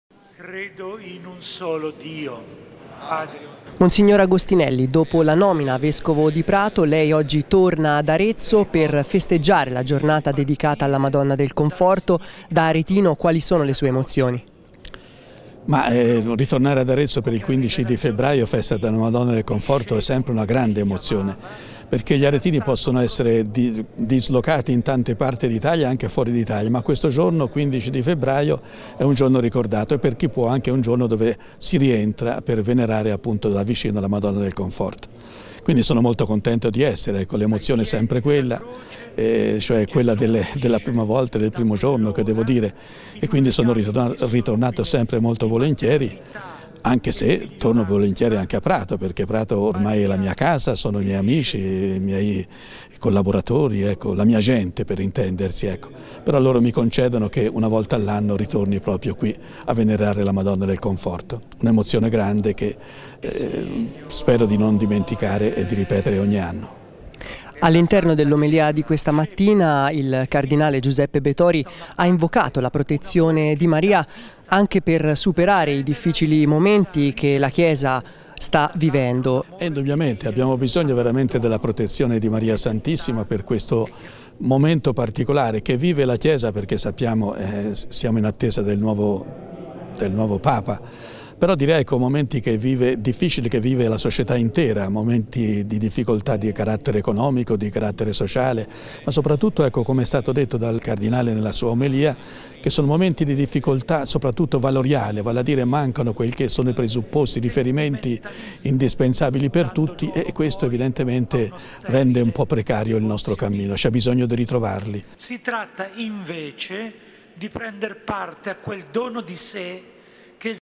Monsignor Agostinelli: "E' sempre un'emozione tornare a casa"
L'intervista a Franco Agostinelli, vescovo di Prato e aretino di origini
Servizio di Riccardo Ciccarelli.
TSD Notizie del 15 febbraio 2013.